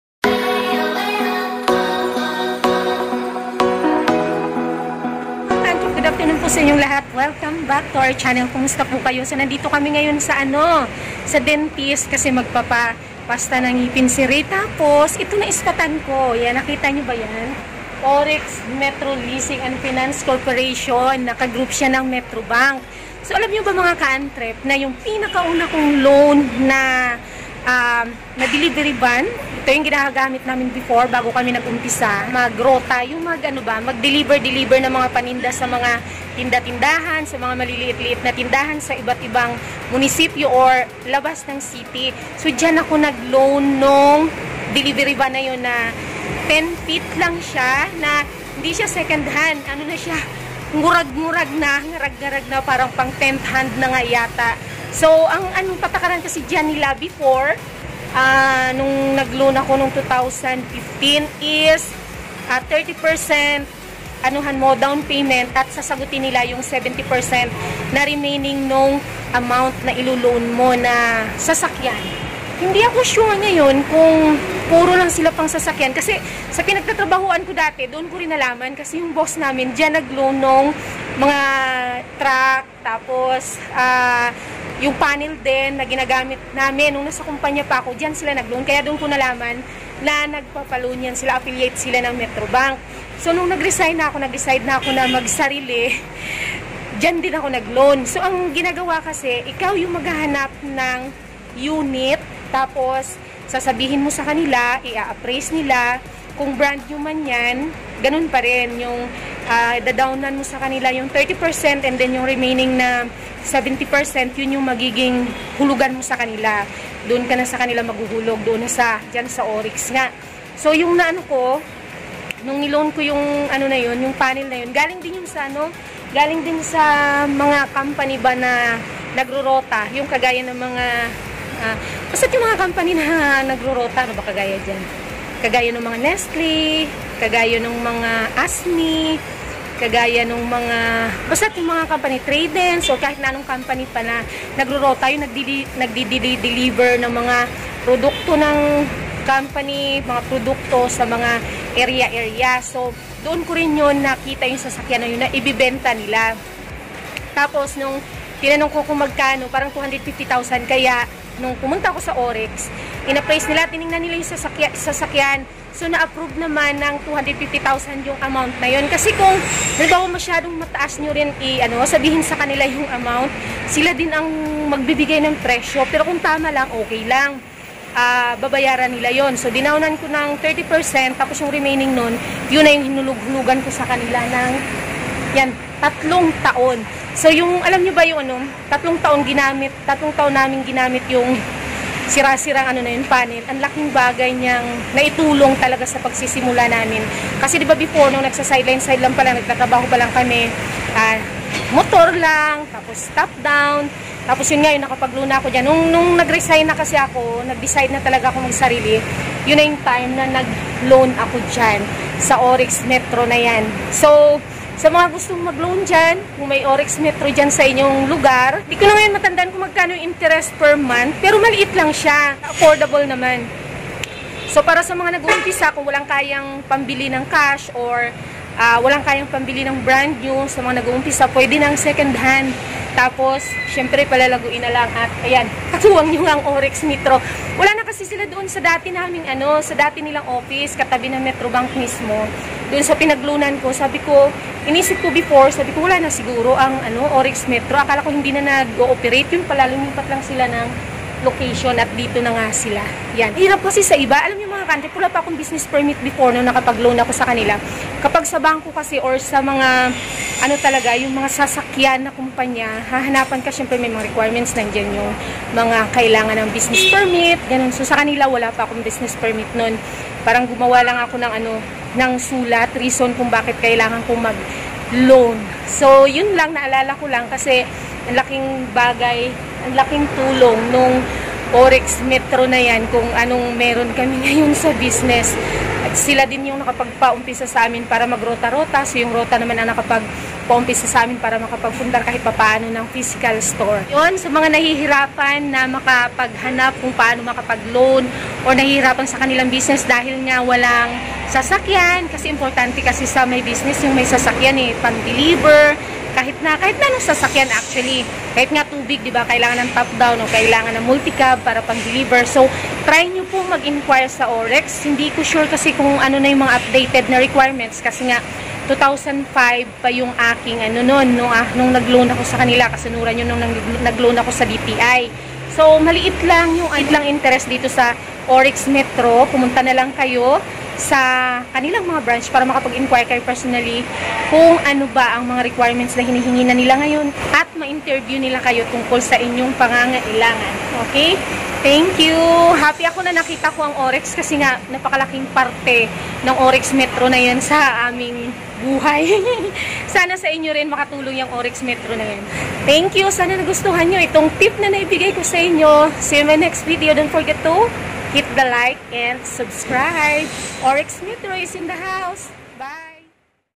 DITO AKO NAG LOAN NG MALIIT NA TRUCK PANG NEGOSYO / MADALI MAG LOAN NG SASAKYAN PANG NEGOSYO
Hello mga ka annetrep, tip po kung gusto mag loan ng sasakyan brand new man or second hand na gagamitin sa negosyo. Check nyo po sa lugar nyo at personal po kayo mag inquire para masagot lahat ng tanong nyo at ma interview na po agad kayo.
paanomagloanng truck
NEGOSYO TIPS
NEGOSYO PHILIPPINES